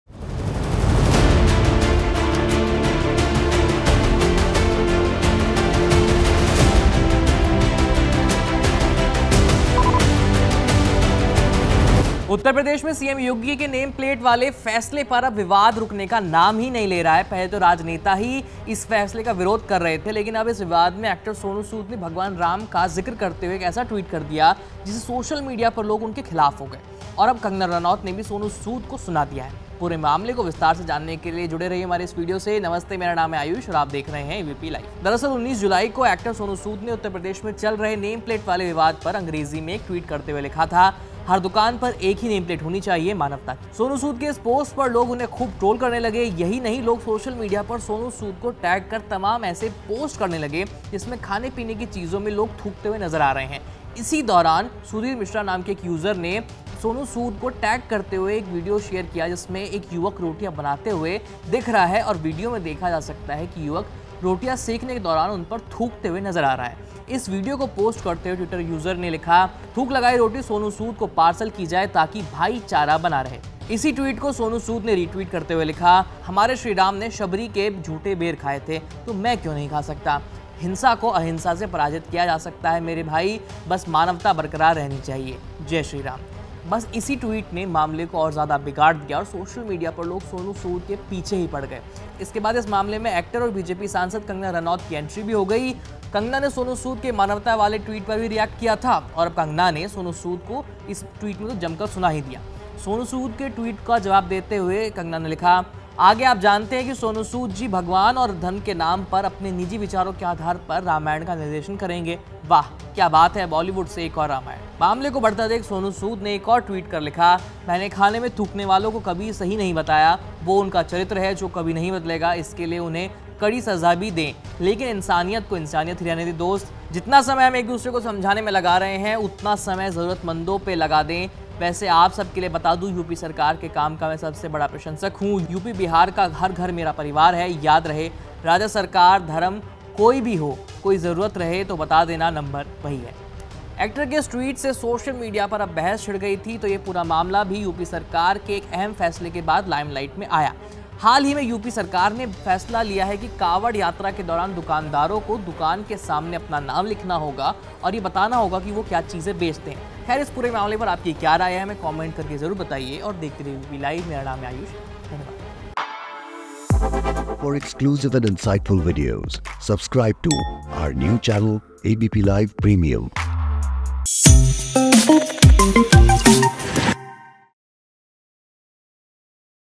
Sonu Sood ने Kanwar Yatra नेम प्लेट विवाद पर ट्वीट किया, Kangna Ranaut क्यों भड़कीं? | ABPLIVE
News:
यूपी में सीएम योगी के कांवड़ यात्रा वाले रास्ते पर मौजूद दुकानों पर नेम प्लेट लगाने वाले फैसले पर विवाद रुकने का नाम ही नहीं ले रहा. अभी तक तो सिर्फ नेता ही उनके इस फैसले का विरोध कर रहे थे, लेकिन अब इस विवाद में एक्टर सोनू सूद ने भगवान राम को जिक्र करते हुए ऐसा ट्वीट कर दिया कि, सोशल मीडिया पर लोग उनके खिलाफ हो गए. और कंगना रनौत ने भी सोनू सूद को सुना दिया. पूरे मामले को विस्तार से जानने के लिए जुड़े रहिए हमारे वीडियो से. नमस्ते मेरा नाम है आयूष और आप देख रहे हैं abp live.

Actor Sonu Sood shared a post on Uttar Pradesh CM Yogi Adityanath's decision on Kanwar Yatra. Sonu Sood also mentioned Shri Ram and Shabari in the name plate controversy. Because of this post, the actor is being trolled a lot on social media. Watch the video to know the news in detail.